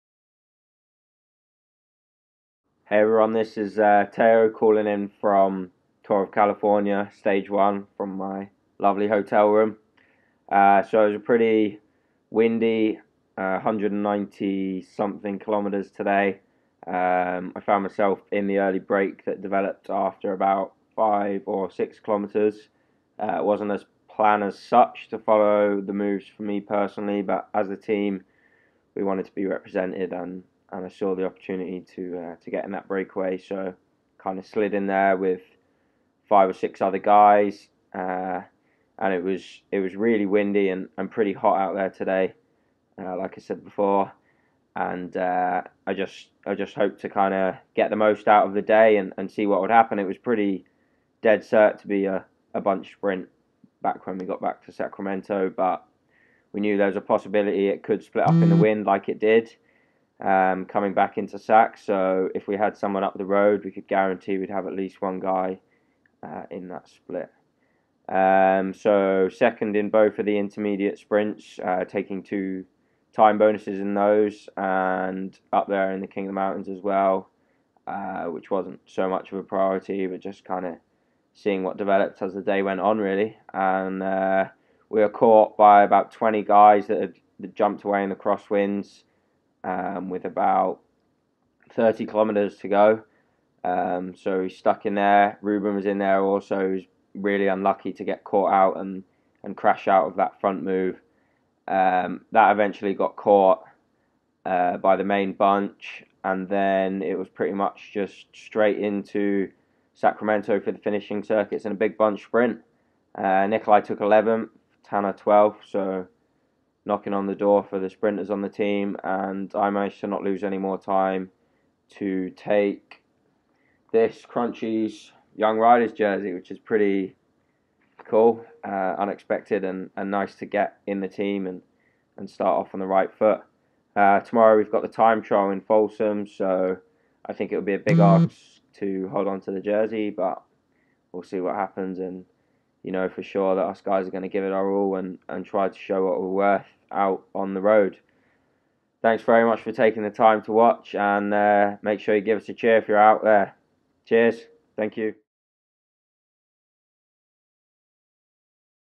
Tao Geoghegan Hart Recaps Stage 1 of The 2014 AMGEN Tour of California
Tao Geoghegan Hart recaps Stage 1 of the AMGEN Tour of California for the Bissell Development Team. Tao took home the Best Young Rider jersey after Stage 1.